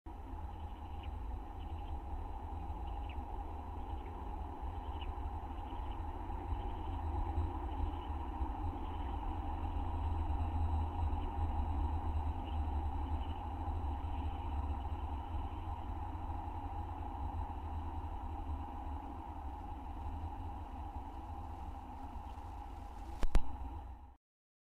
0729 confirmed #6 still whole but possible pip.avi
Okay its not a hatch but it IS a possible pip! this is day 33